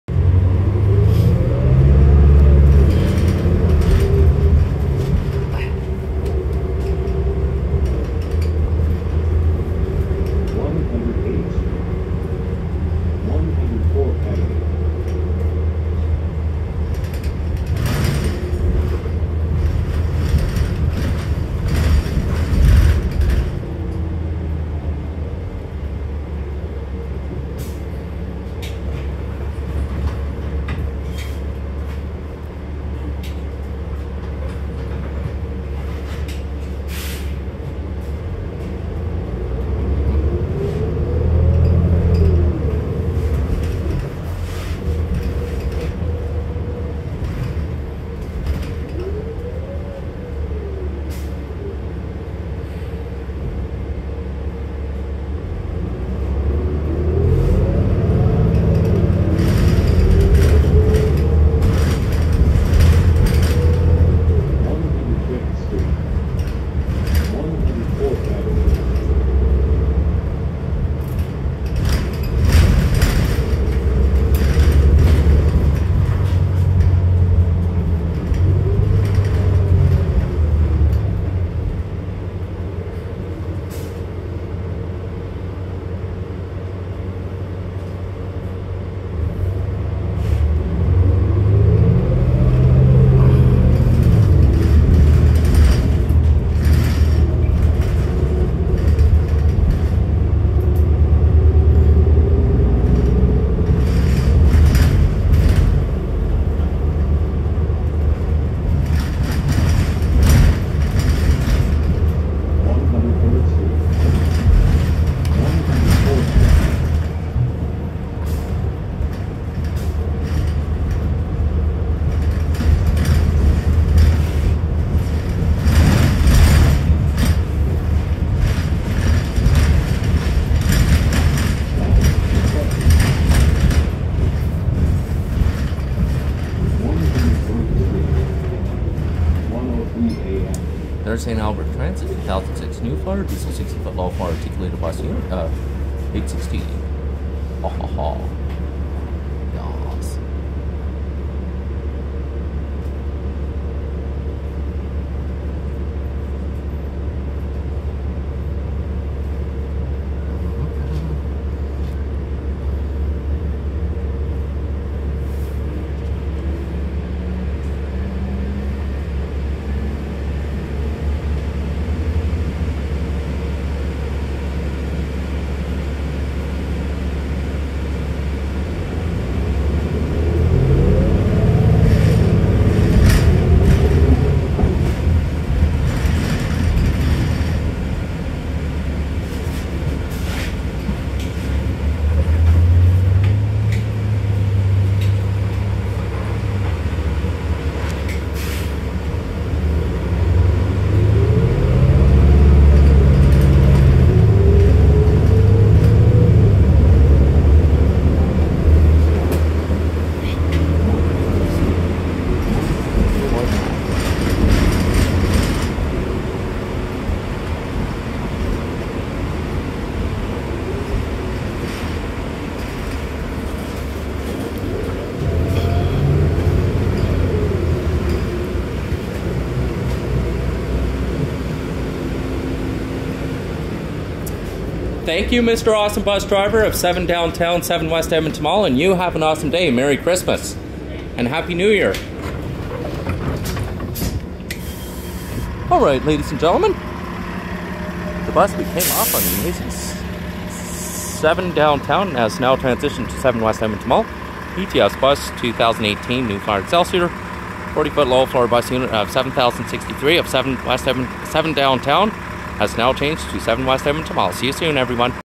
ETS Bus 2018 New Flyer XD40 #7063 On 7 Downtown
From 108th Street & 104th Ave, By MacEwan University To 100th Street & 104th Ave, Outside CN Office Tower, Royal Alberta Museum, in The Downtown Area in Edmonton Alberta.
December 18th 2023!